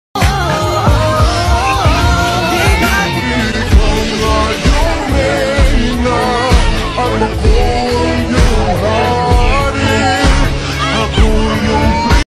BestCelebrityEncounters
Wrap up parties is the time to be silly, friendly, and having fun.

Dancin' with Jared Turner, a New Zealand actor who will be playing Todd Carpenter in Almost Paradise.

Almost Paradise is airing 8:45PM in Kapamilya Channel, Kapamilya Online Live, A2Z11 and Iwanttfc!